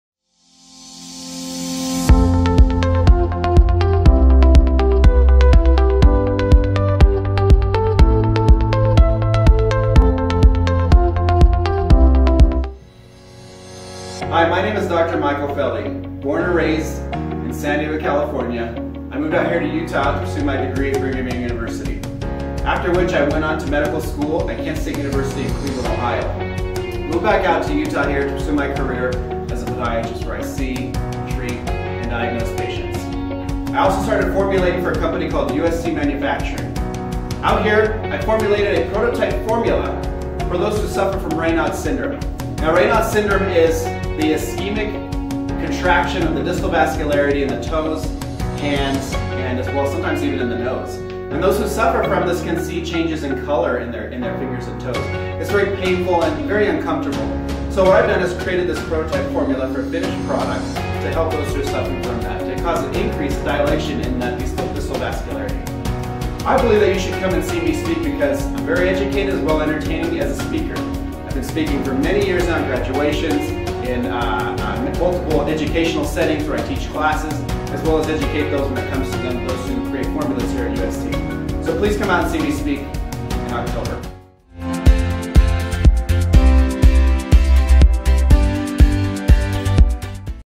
The Phenomenon we call Raynaud’s by Dr Michael Felley
@3rd International Conference on Integrative Medicine and Alternative treatments October 26-27, 2018 Boston | Massachusetts | USA.

The Phenomenon we call Raynaud’s

Speech on Reynaud’s Syndrome which is a frustrating condition in which people suffer when the vasculature in their distal extremities constricts By Michael Felley, Podiatric Medicine, USA